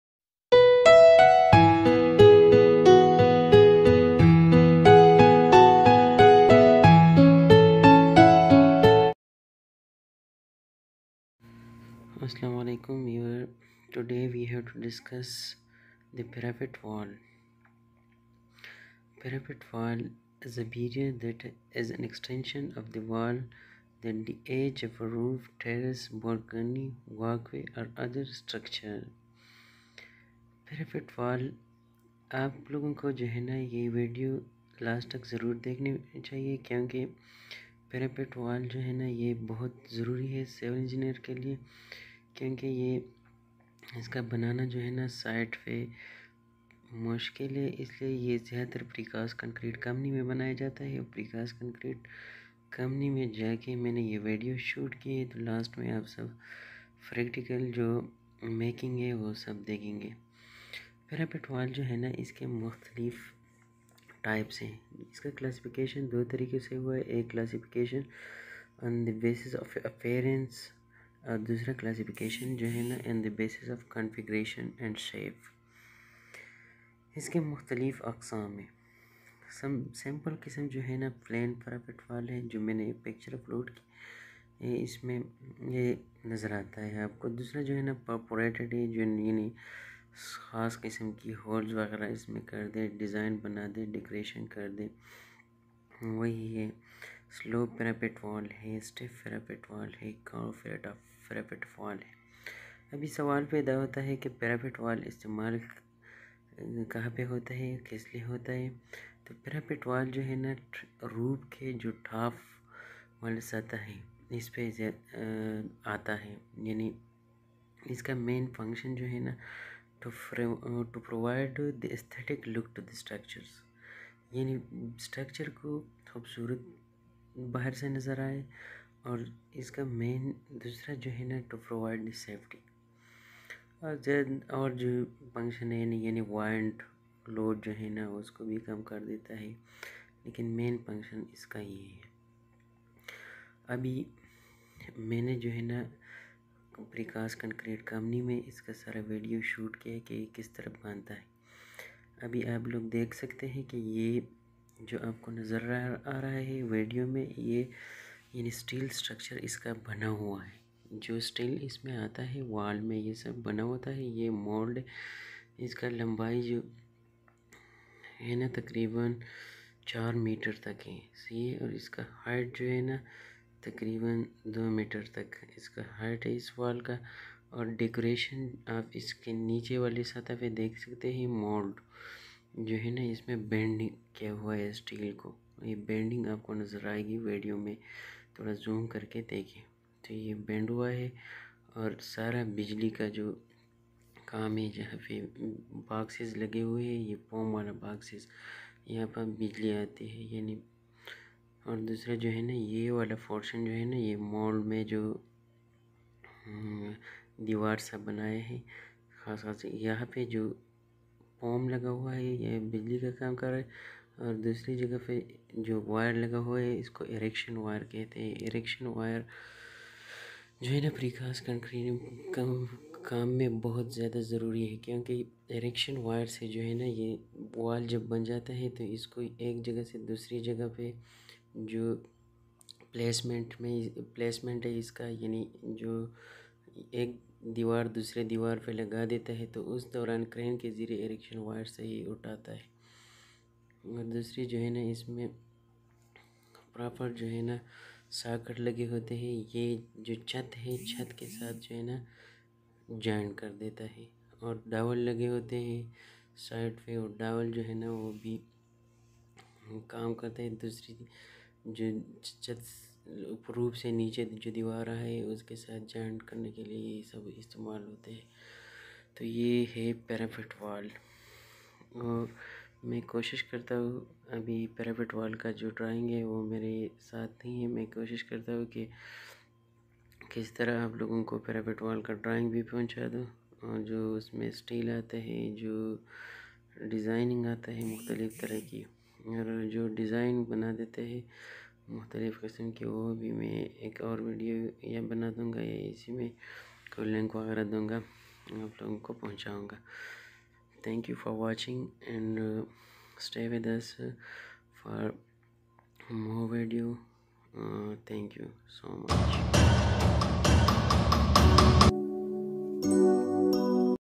Parapet wall construction in precast concrete company
Parapet wall construction on site parapet  wall is a barrier that is an extension of the wall at the edge of a roof ,terrace  balcony, walk way or other  structure there are different types of Parapet wall
1) plain Parapet wall
2) perforated Parapet wall
3) slop Parapet wall
4) stepped Parapet wall
5)curved Parapet wall